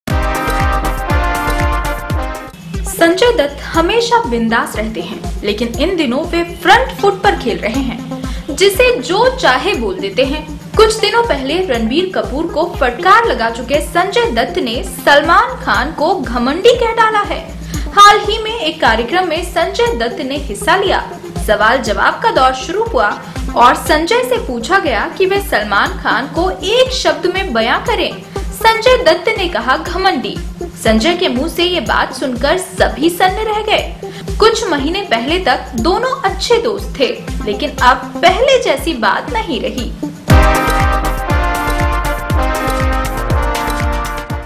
संजय दत्त ने सलमान खान को कहा 'घमंडी'
संजय दत्त हमेशा बिंदास रहे हैं, लेकिन इन दिनों तो वे फ्रंट फुट पर खेल रहे हैं। जिसे जो चाहे वो बोल रहे हैं। कुछ दिनों पहले रणबीर कपूर को फटकार लगा चुके संजय दत्त ने सलमान खान को 'घमंडी' कह डाला है। संजय से पूछा गया कि वे सलमान खान को एक शब्द में बयां करें। संजय दत्त ने कहा 'घमंडी'।